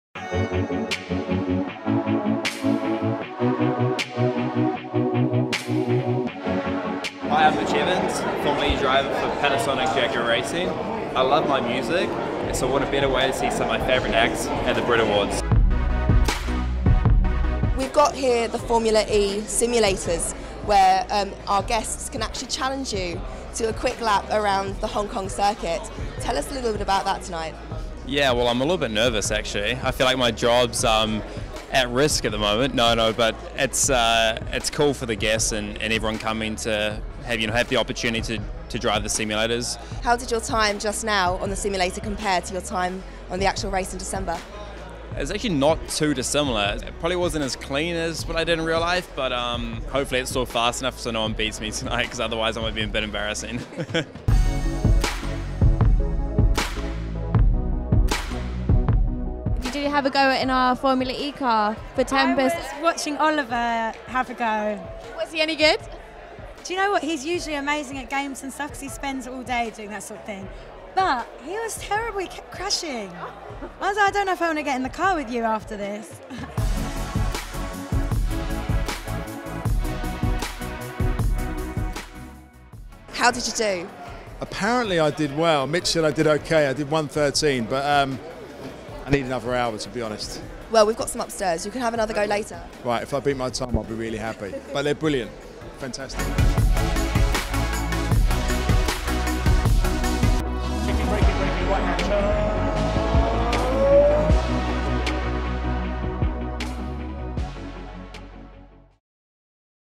Formula E star Mitch Evans challenges celebrities to a race at the Tempus BRITs Party
When Formula E driver Mitch Evans heard Tempus was inviting a host of stars to the BRITs Official Aftershow party in our role as Headline Partner, he couldn't wait to mix the worlds of racing and music. Evans challenged Pixie Lott, Oliver Cheshire, Tallia Storm and Paul Sculfor to a race around the Hong Kong circuit on their official simulators. Find out how they did in our exclusive video.
Visit tempusmagazine.co.uk/brits for more of our exclusive awards coverage